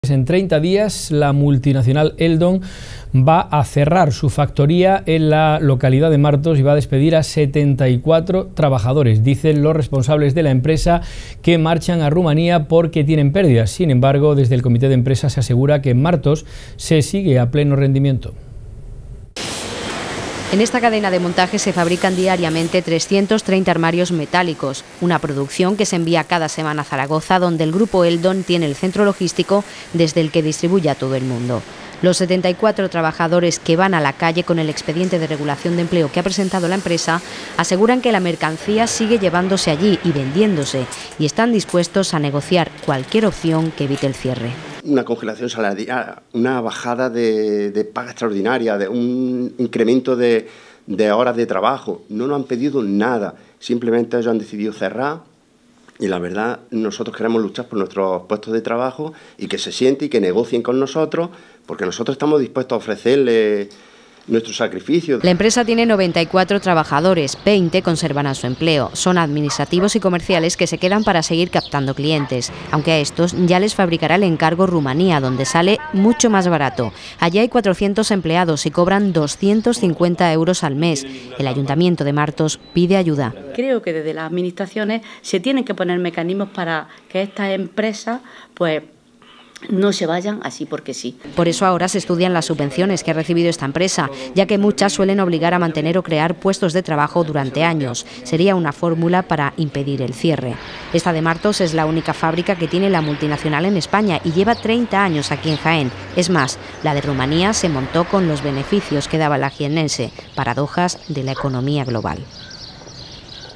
ELDON PREVÉ 74 DESPIDOS EN MARTOS
En 30 días, la multinacional Eldon, tiene previsto cerrar su fábrica de Martos y despedir a 74 trabajadores. Ha anunciado un ERE porque traslada su producción a Rumanía. Dice que tiene pérdidas aunque el comité de empresa asegura que siguen trabajando a pleno rendimiento.